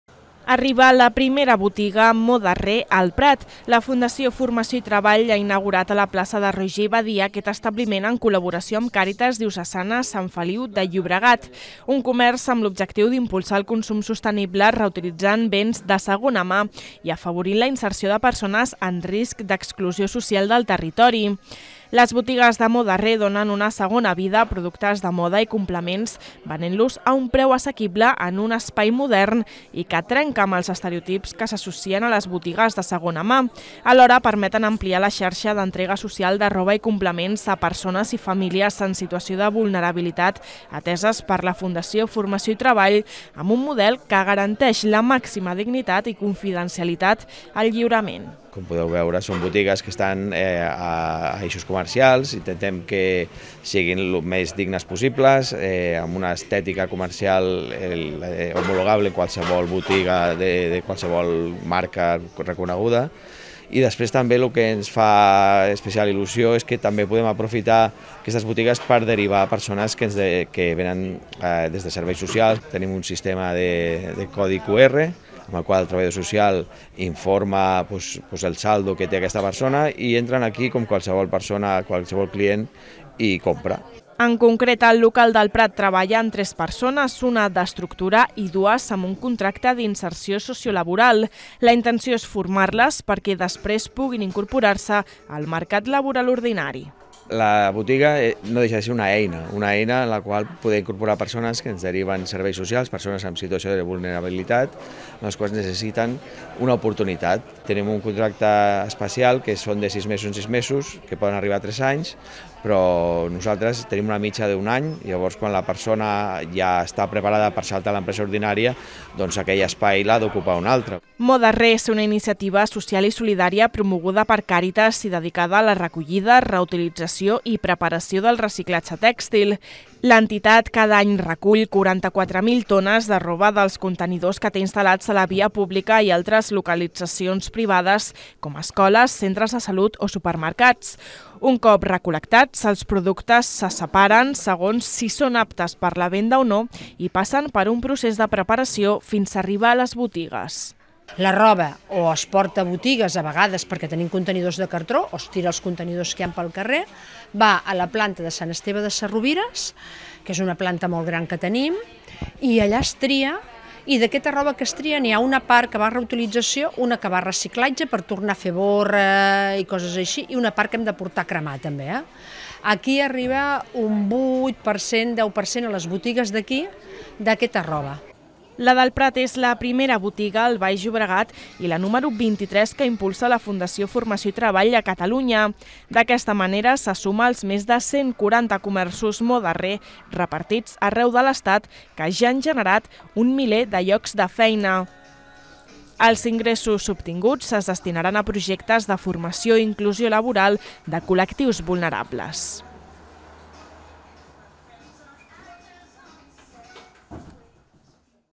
Obre la primera botiga Moda re- al Prat
La Fundació Formació i Treball ha inaugurat a la Plaça de Roigé i Badia aquest establiment en col·laboració amb Càritas Diocesana Sant Feliu de Llobregat. Un comerç amb l’objectiu d’impulsar el consum sostenible reutilitzant béns de segona mà i afavorint la inserció de persones en risc d’exclusió social del territori.